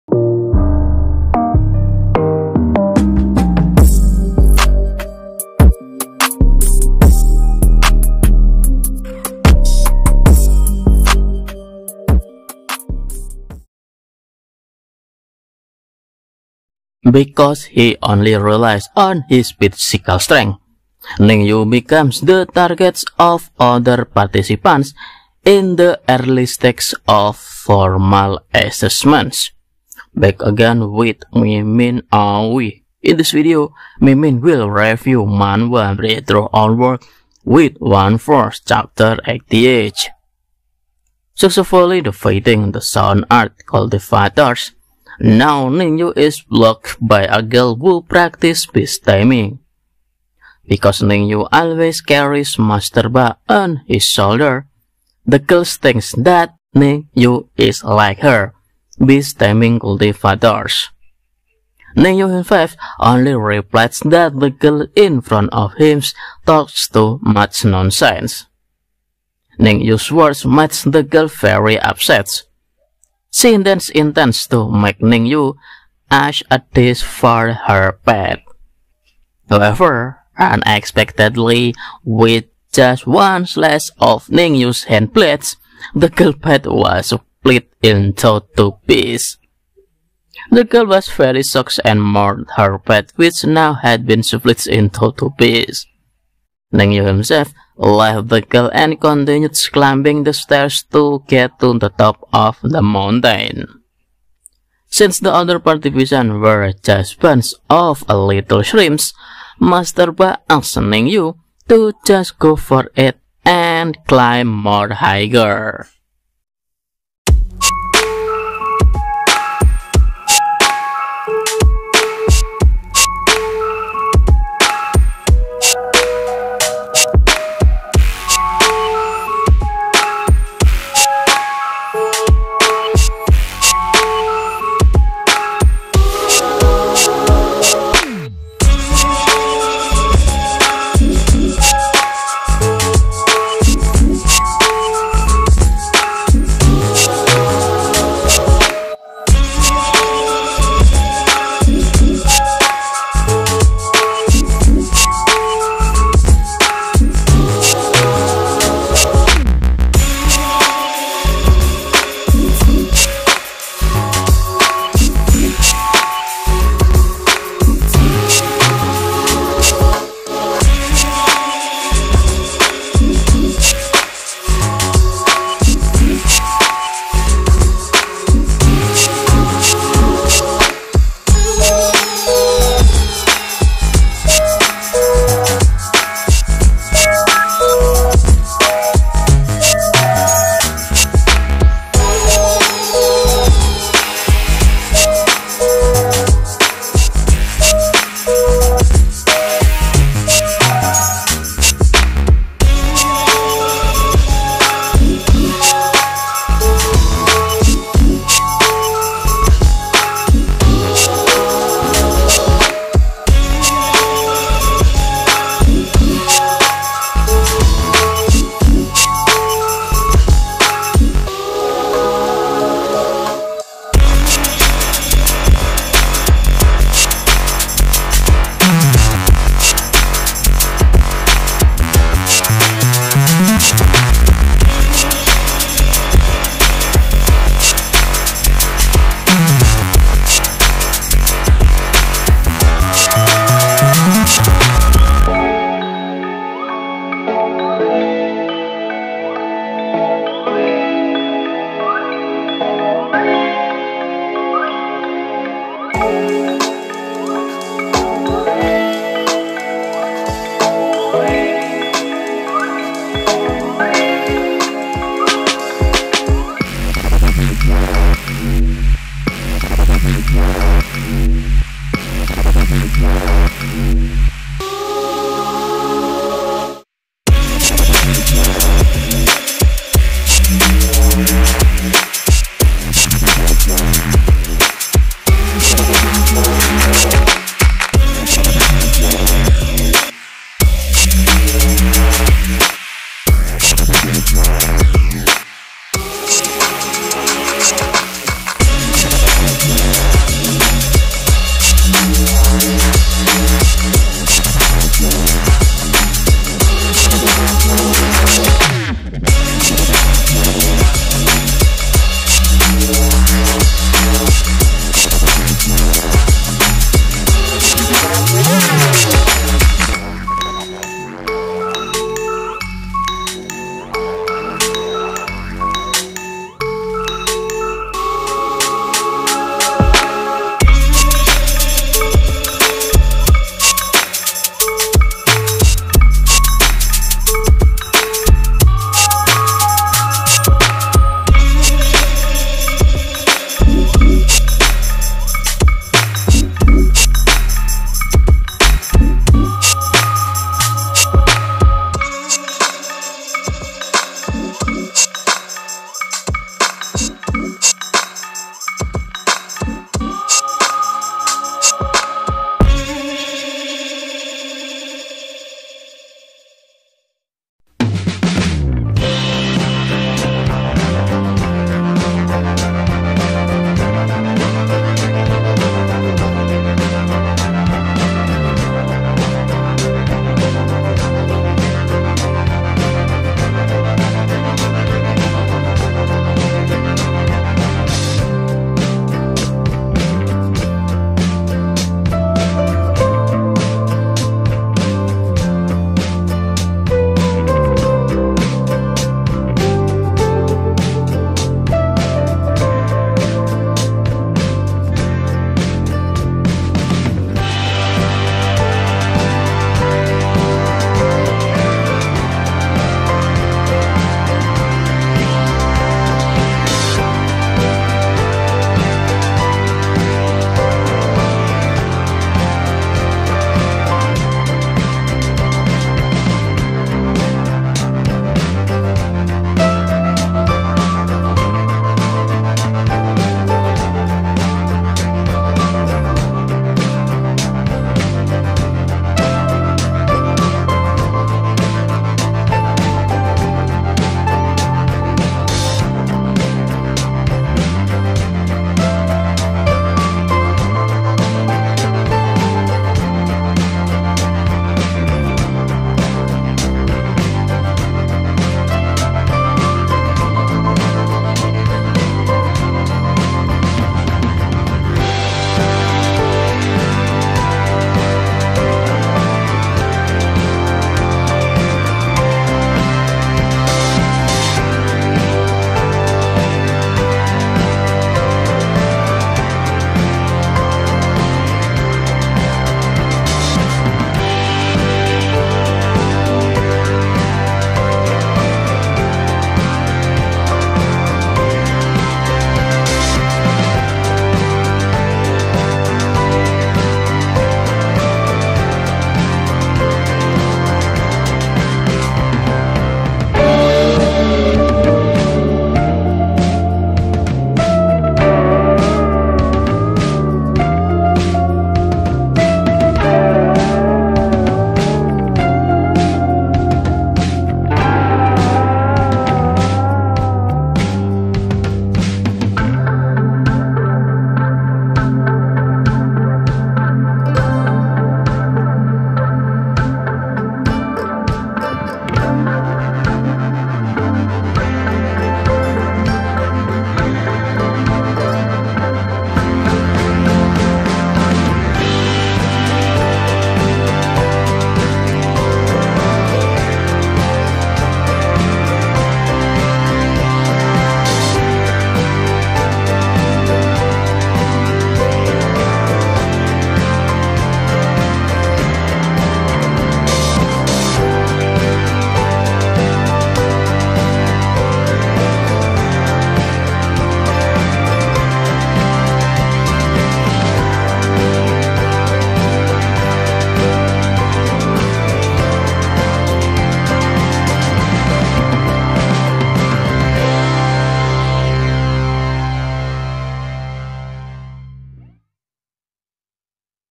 Dark Horse ™ Breakthrough All Worlds With One Force Chapter 88
Ning Yu that only relies oj his physical strength, manage to climb higher than other participants.

 COPYRIGHT / LICENSING